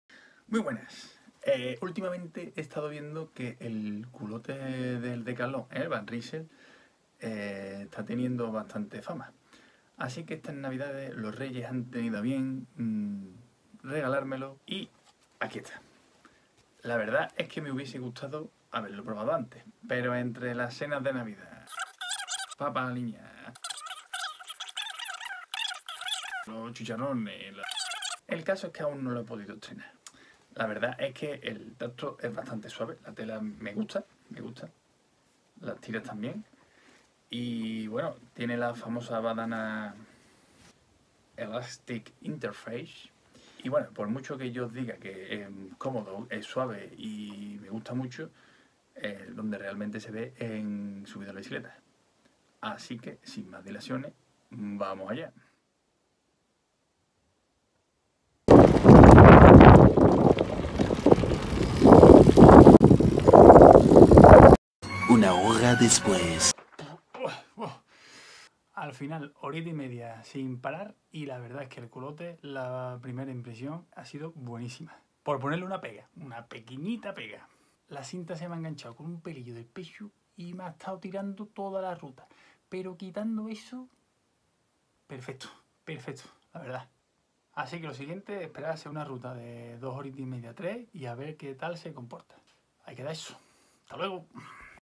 CULOTTE VAN RYSEL - PRIMERA IMPRESIÓN
Bueno, pues después de oír maravillas sobre esta culotte he decidido probarlo a ver qué tal va